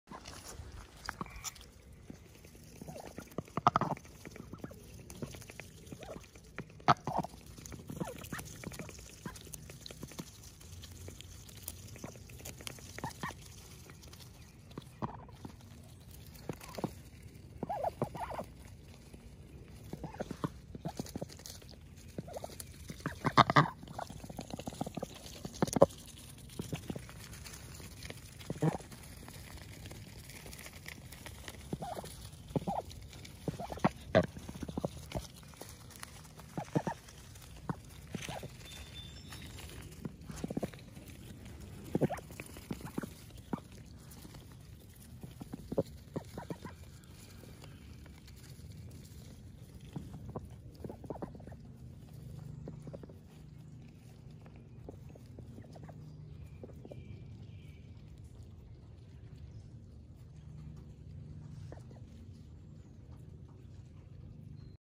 Satoshi and Tokyo the Jumbo Pekin Ducks - Munchin' on Weeds - May 17, 2021
Satoshi and Tokyo are two Jumbo Pekin Ducks. They were born at 7AM on December 28th and are 20 weeks old. Here's a video of them munching on some weeds.